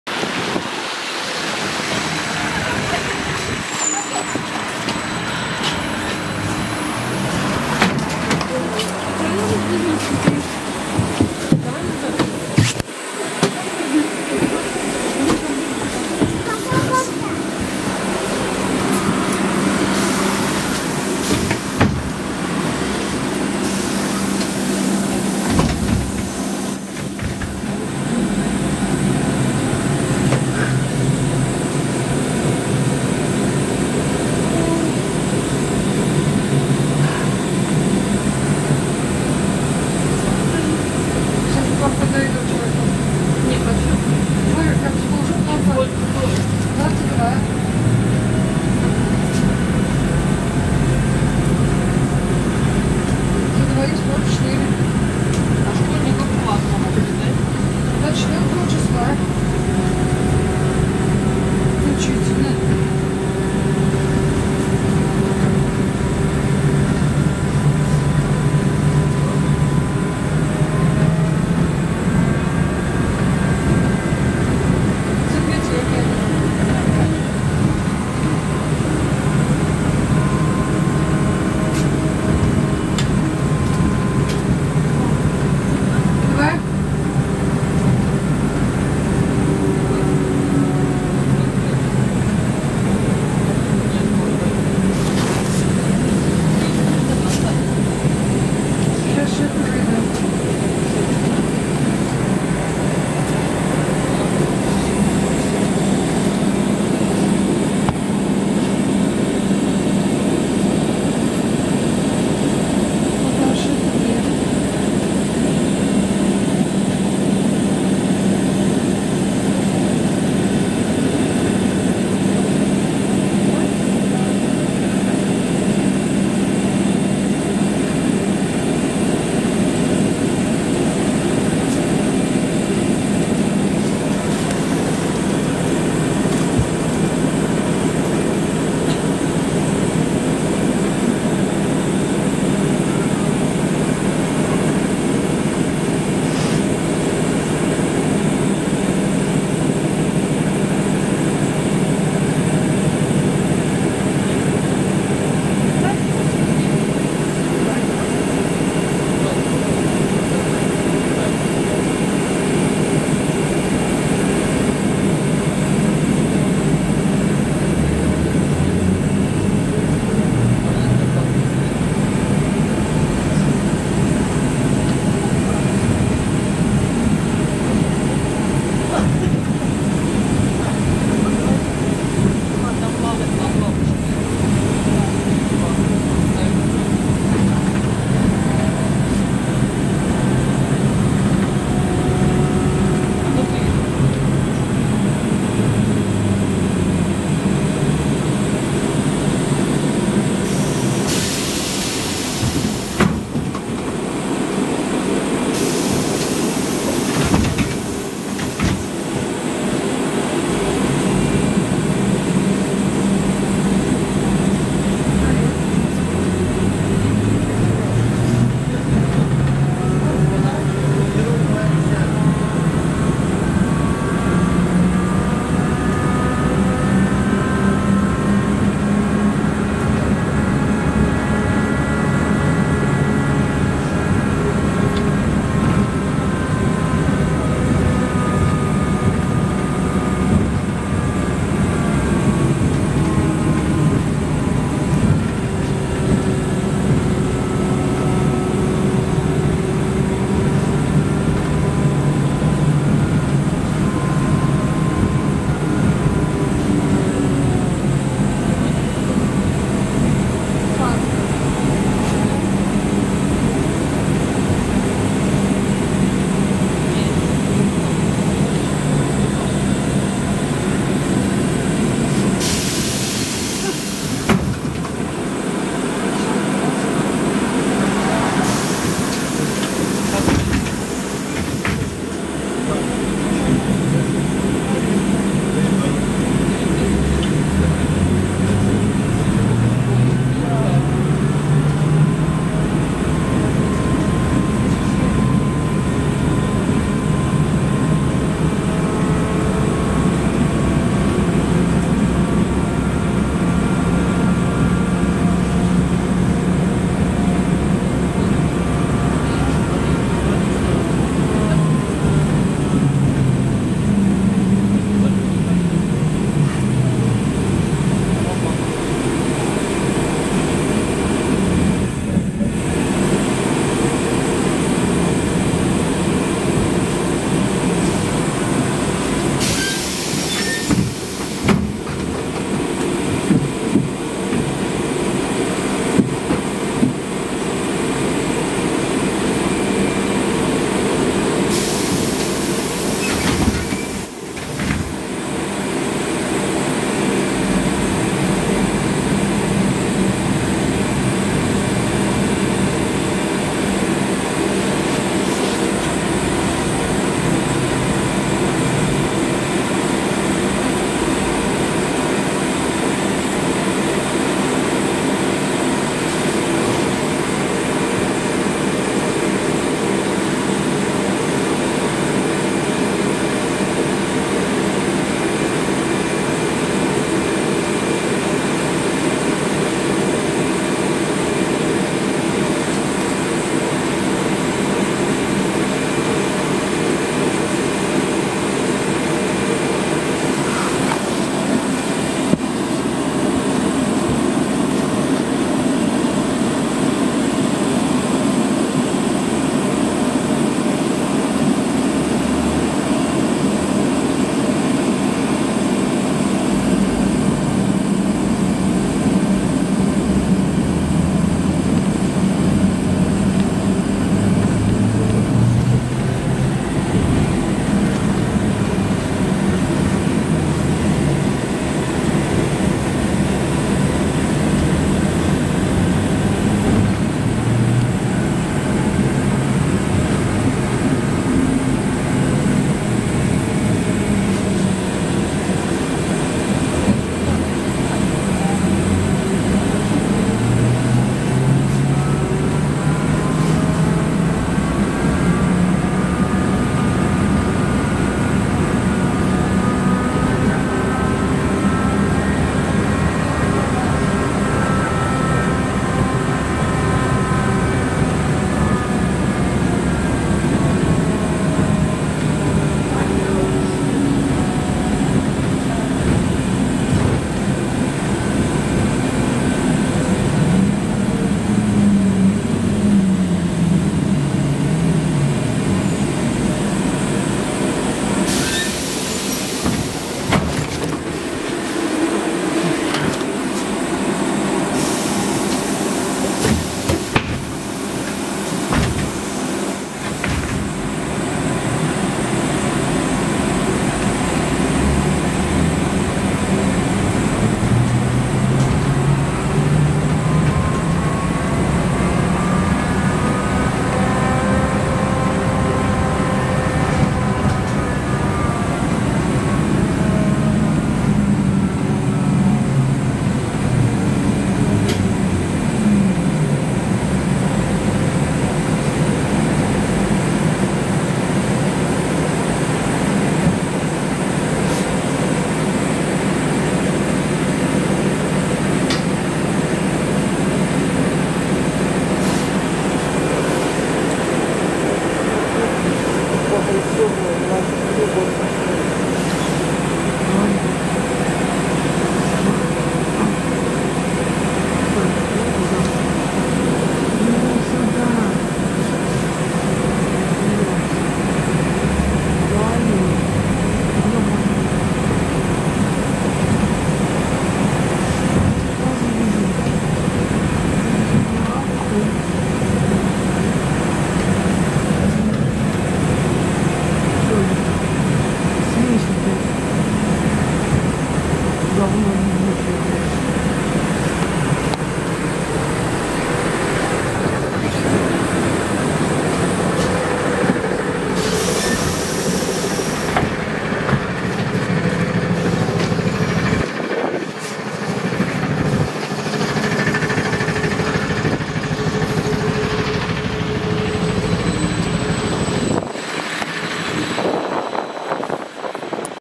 Поездка на троллейбусе Тролза-5275.03 «Оптима» № 19 по маршруту №15 в Ульяновске . (14.11.2021)
Время съемки : 13 ‎ноября ‎2021 ‎г., ‏‎18:26:40
Участок : Детская поликлиника № 6 - Микрорайон Алексеевка
Автор видео - Марк Вассаев
Модель камеры Canon PowerShot SX 540 HS